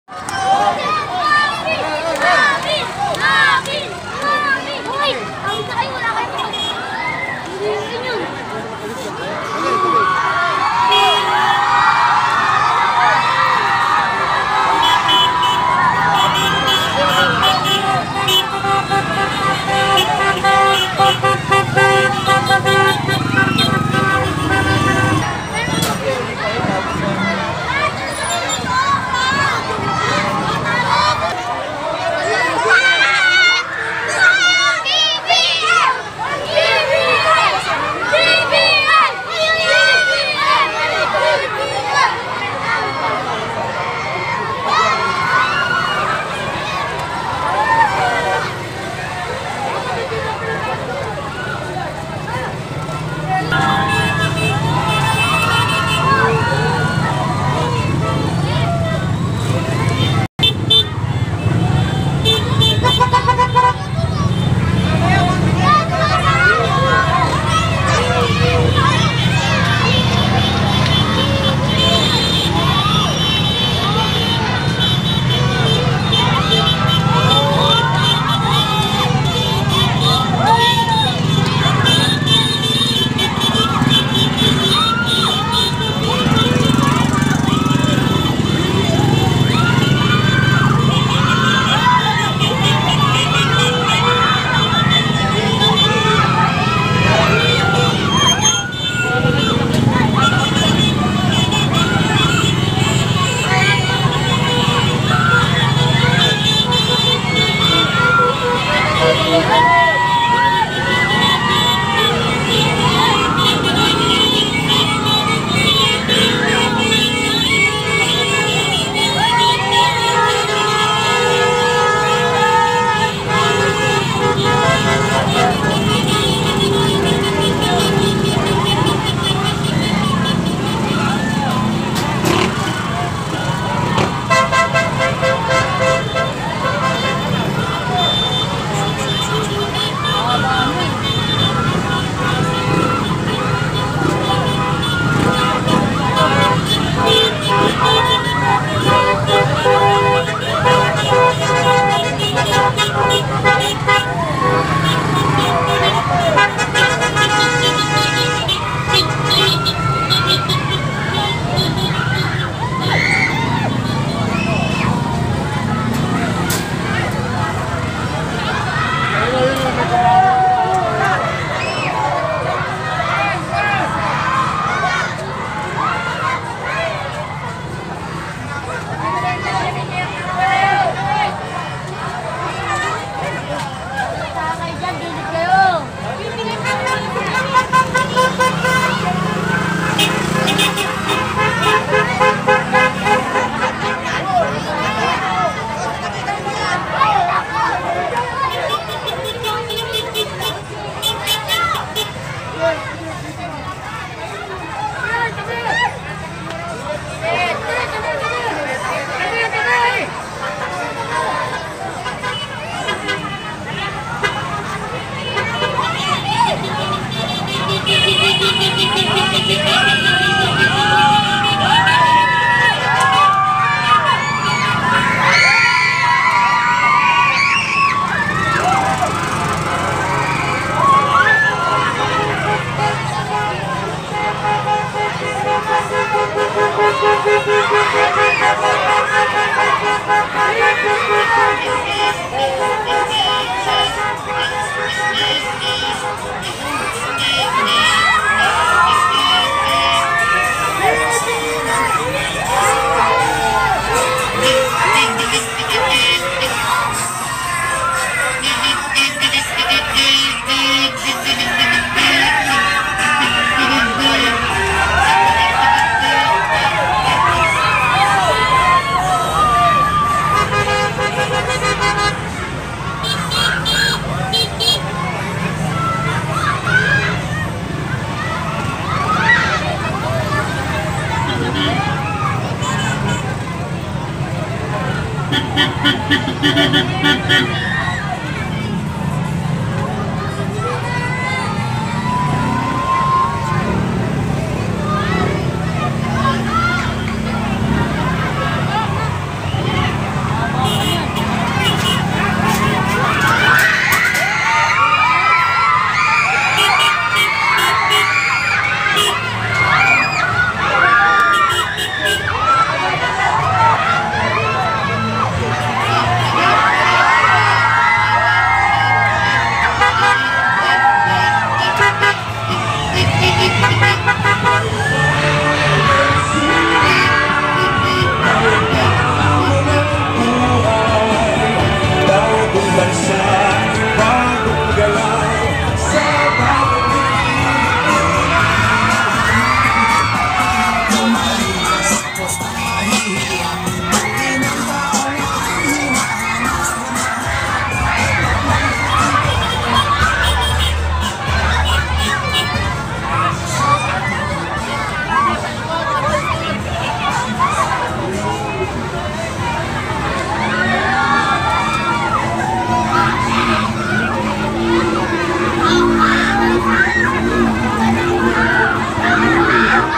BBM-SARA UNITEAM sa Bagong Silang-Caloocan North: Part 2
Mainit na inabangan at sinalubong ng Caloocan City ang Uniteam BBM-SARA